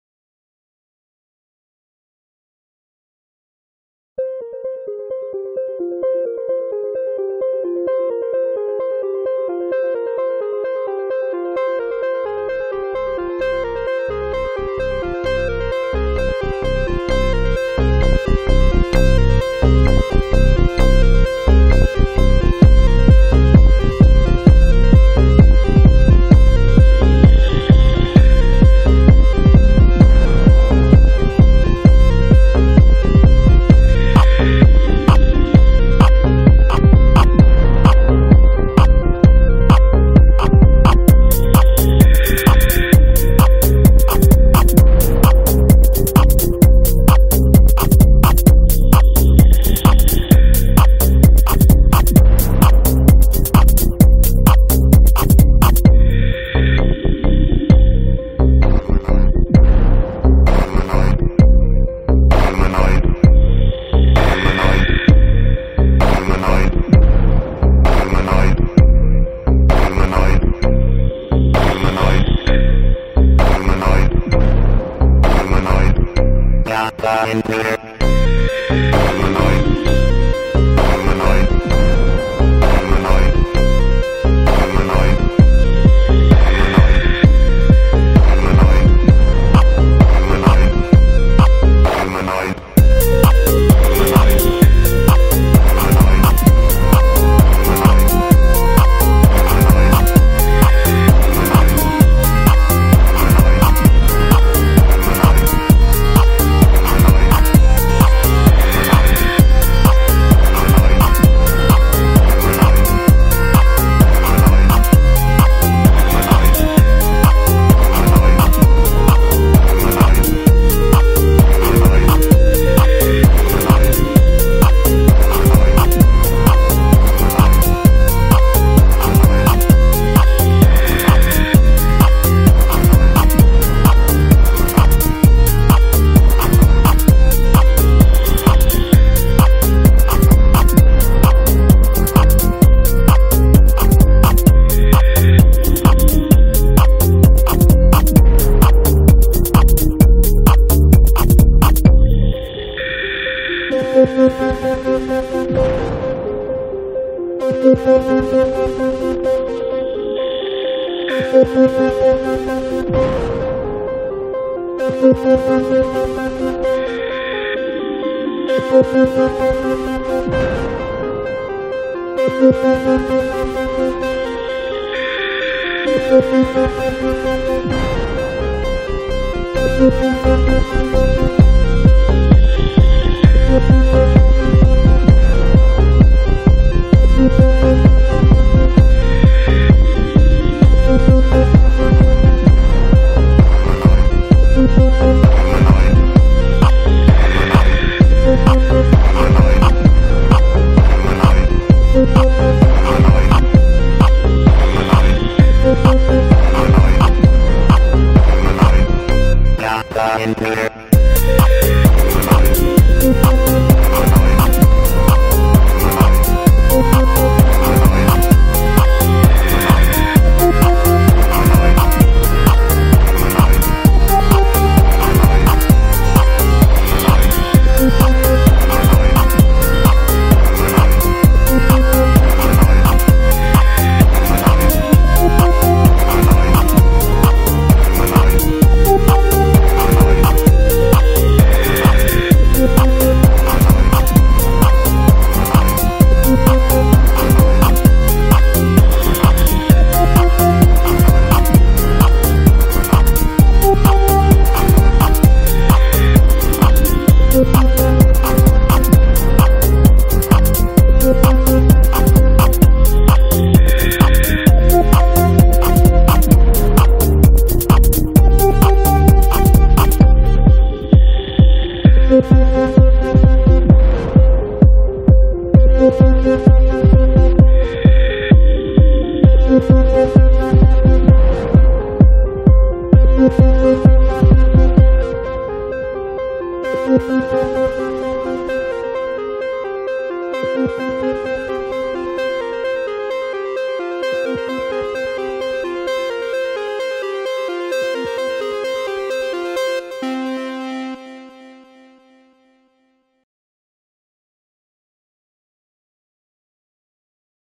Fraktalzoom: Glax
Ein langsamer Zoom in die Mandelbrotmenge zu monotoner, repetitiver Computermusik (natürlich wieder von mir), mehr nicht.